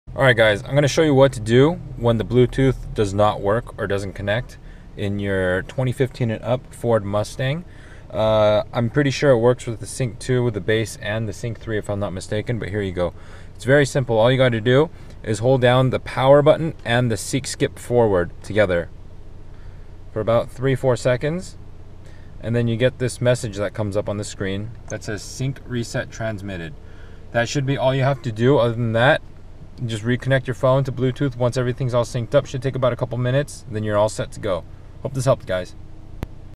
Fix Ford Microsoft Sync, Phone, Bluetooth Radio Issues and Disconnections
I didn't find any videos on how to do this on youtube, so I thought I would go out and make one really quickly. This works on all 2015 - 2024 Ford Mustang. V6, Ecoboost, GT, GT350, and GT500. It also works on all version of Sync. Sync 2 and Sync 3. F150, Explorer, and other models.